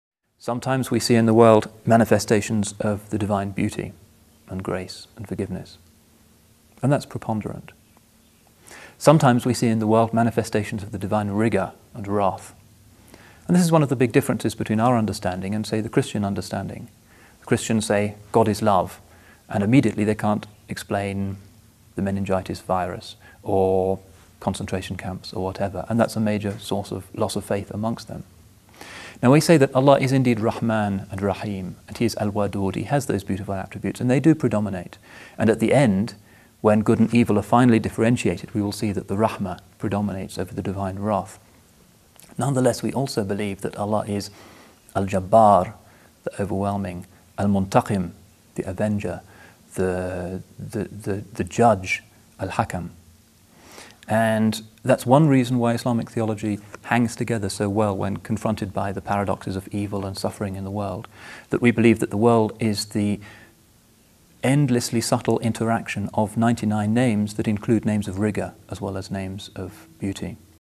Allah Has Names Of Beauty & Rigour - Shaykh Abdal-Hakim Murad
"Allah has names of Beauty: the Compassionate, the Merciful, the Gentle, and many others. But He also has Names of Rigour: the Overwhelming, the Just, the Avenger. The world in which we live exists as the interaction and the manifestation of all of the divine attributes. Hence it is a place of ease and of hardship, of joy and of sorrow. It has to be this way: a world in which there was only ease could not be a place in which we can discover ourselves to be true human beings. It is only by experiencing hardship, and loss, and bereavement, and disease, that we rise above our egos, and show that we can live for others, and for principles, rather than only for ourselves."

Shaykh Abdal-Hakim Murad

📽 Reliance On Allah: The Cure For An Ummah In Crisis by Shaykh Abdal-Hakim Murad

📝 When The Generous Appears With The Name Avenger by Shaykh Abdal-Hakim Murad